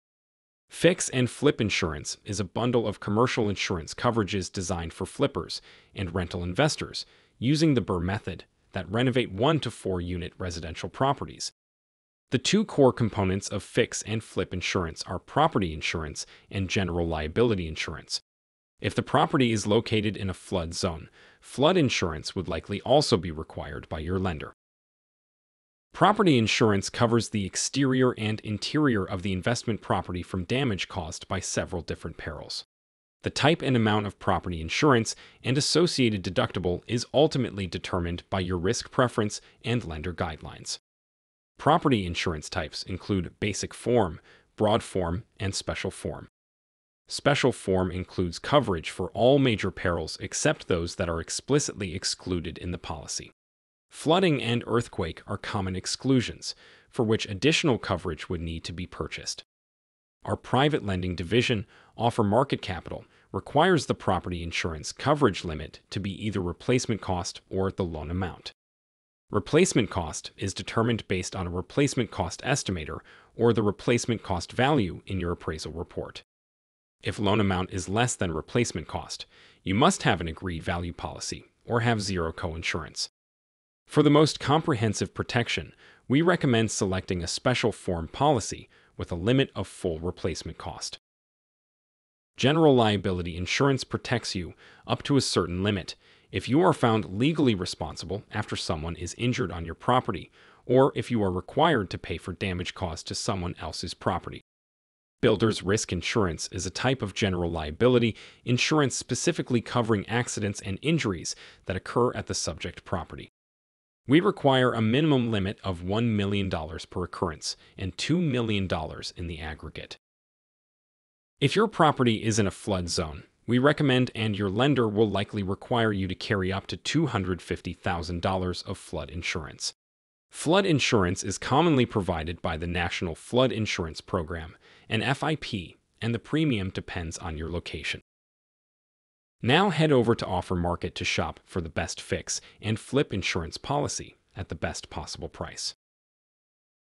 Fix & Flip Builder's Risk Insurance
Fix and flip insurance is a bundle of commercial insurance coverages designed for flippers, and rental investors using the BRRR method, that renovate 1 to 4 unit residential properties.

The two core components of fix and flip insurance are Property Insurance and General Liability Insurance. If the property is located in a flood zone, Flood Insurance would likely also be required by your lender.

Property insurance covers the exterior and interior of the investment property from damage caused by several different perils. The type and amount of property insurance and associated deductible is ultimately determined by your risk preference and lender guidelines.

Property insurance types include "Basic Form", "Broad Form" and "Special Form".

Special Form includes coverage for all major perils except those that are explicitly excluded in the policy. Flooding and earthquake are common exclusions, for which additional coverage would need to be purchased.

Our private lending division, OfferMarket Capital, requires the property insurance coverage limit to be either Replacement Cost or the Loan Amount.

Replacement Cost is determined based on a Replacement Cost Estimator or the replacement cost value in your appraisal report.

If loan amount is less than Replacement Cost, you must have an Agreed Value Policy or have zero coinsurance.

For the most comprehensive protection, we recommend selecting a Special Form policy with a limit of full Replacement Cost.

General liability insurance protects you, up to a certain limit, if you are found legally responsible after someone is injured on your property or if you are required to pay for damage caused to someone else's property.

Builder's Risk Insurance is a type of General Liability insurance specifically covering accidents and injuries that occur at the subject property.

We require a minimum limit of $1,000,000 per occurrence and $2,000,000 in the aggregate.

If your property is in a flood zone, we recommend and your lender will likely require you to carry up to $250,000 of flood insurance. Flood insurance is commonly provided by the National Flood Insurance Program (NFIP) and the premium depends on your location.

Now head over to OfferMarket to shop for the best fix and flip insurance policy, at the best possible price: offermarket.us/insurance

Our mission is to help you build wealth through real estate.